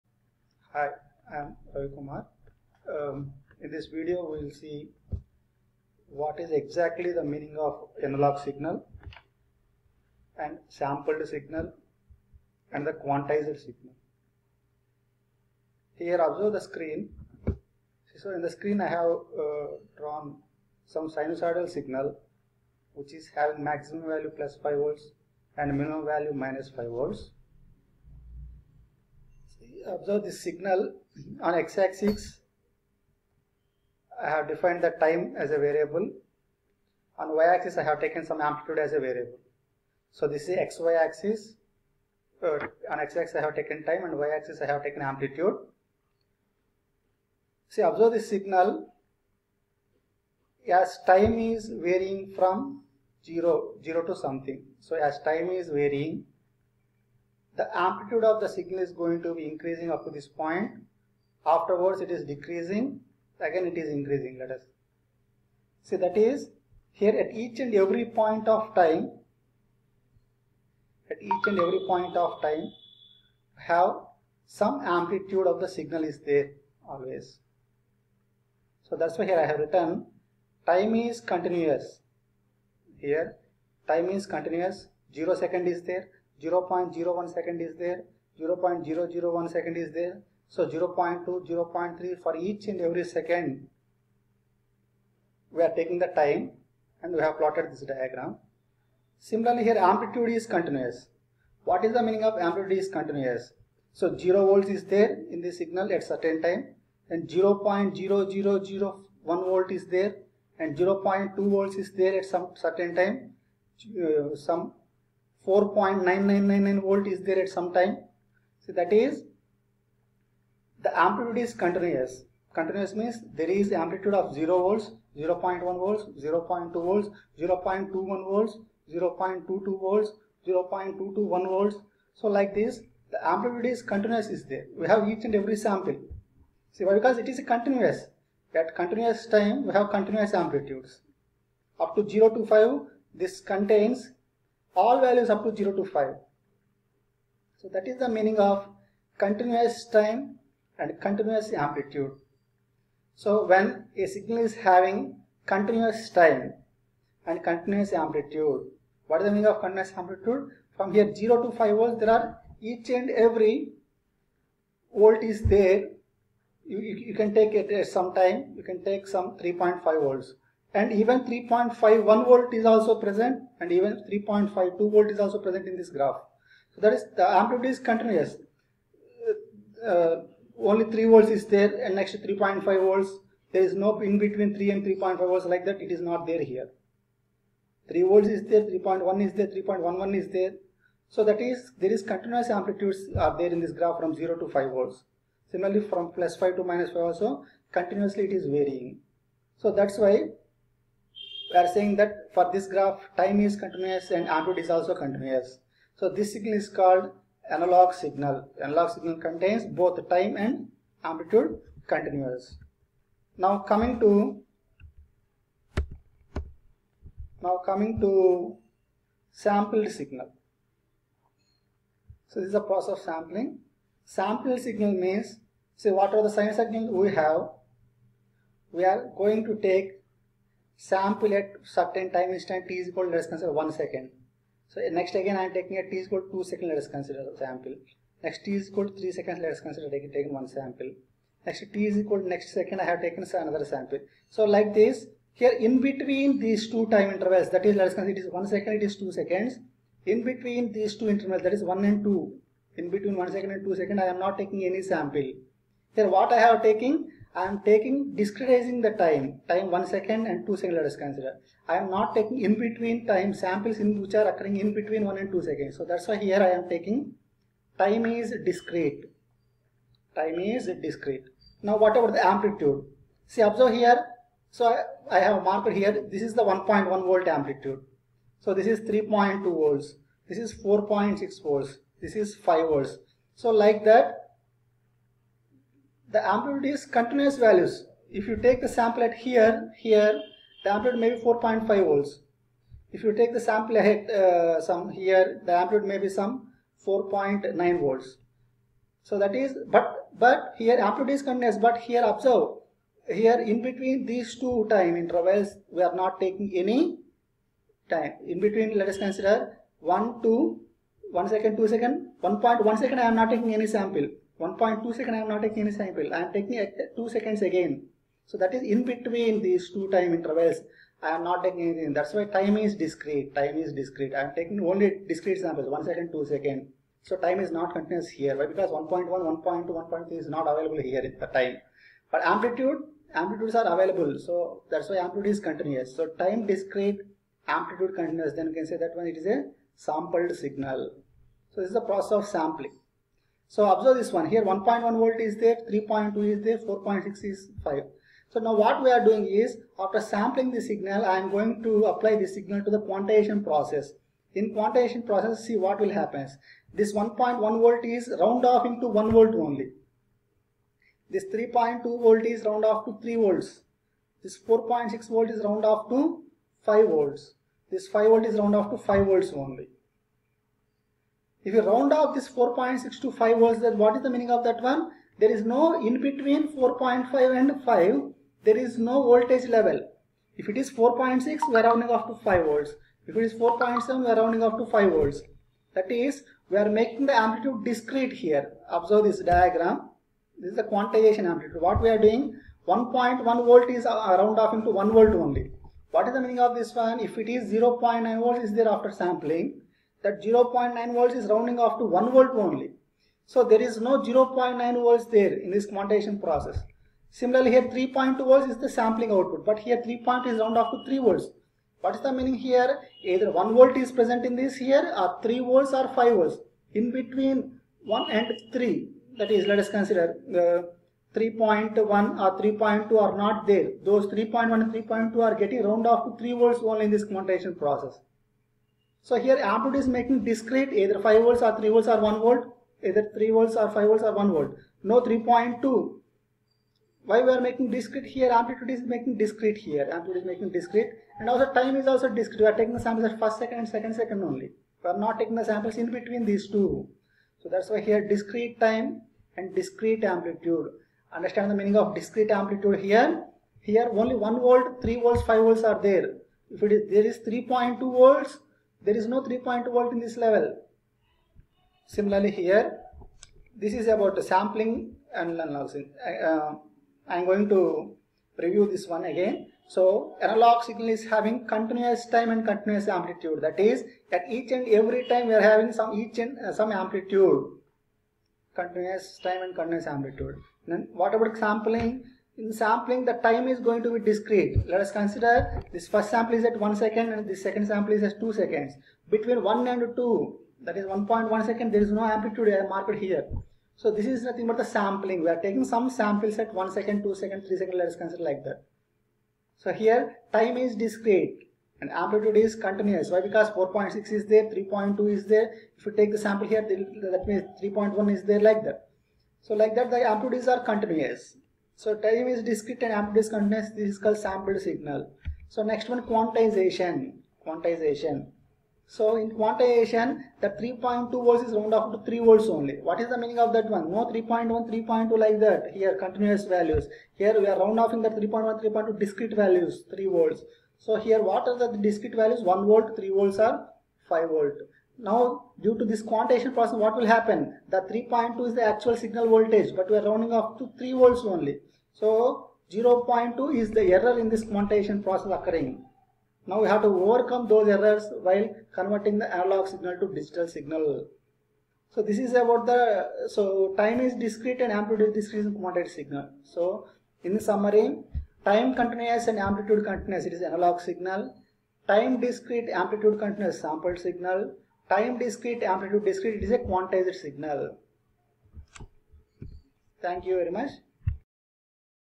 Continuous Time Continuous Amplitude, Discrete time continuous amplitude, Discrete time and Discrete
Difference between Analog signal, Sampled Signal and Quantized Signal